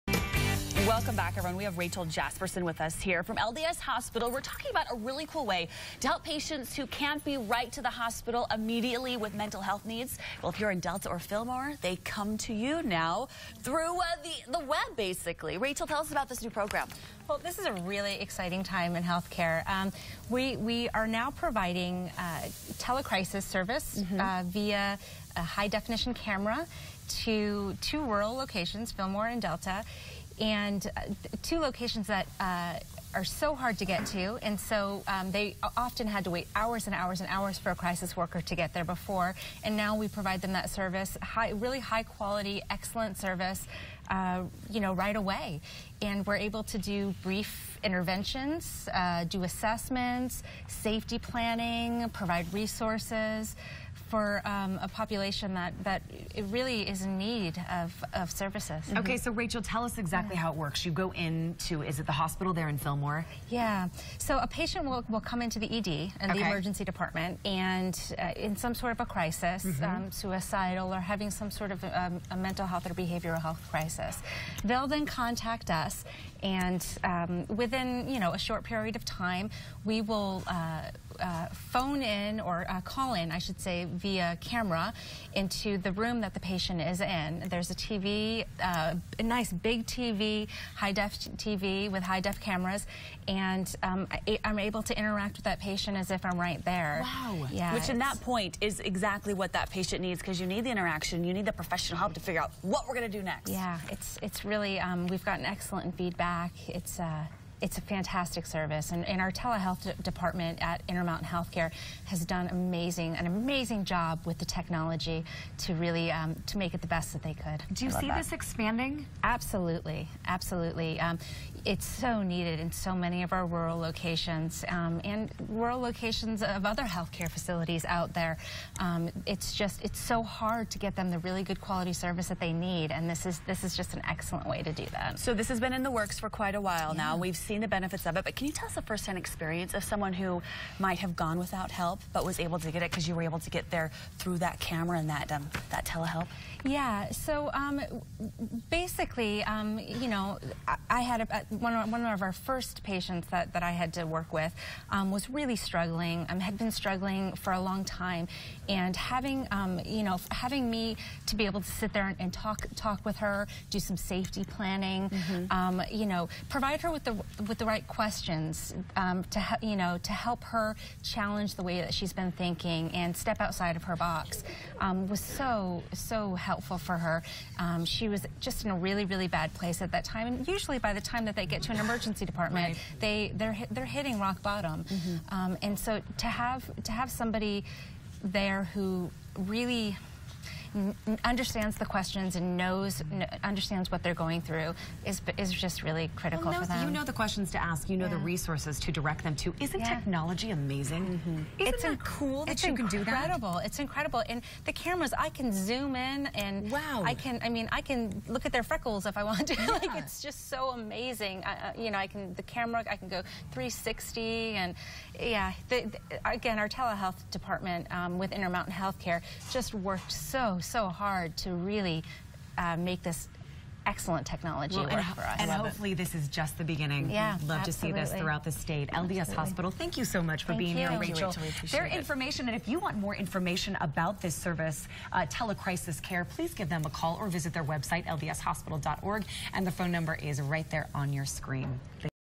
Fast Help For Mental Crises
LDS Hospital has launched a new service called TeleCrisis Care. This service allows mental health professionals at LDS Hospital to quickly connect with patients who are in crisis and need mental health services at Delta Community Medical Center and Fillmore Community Medical Center, 24 hours a day, seven days a week. Rachael Jasperson from LDS Hospital speaks with the ladies at Good for Utah about this new service.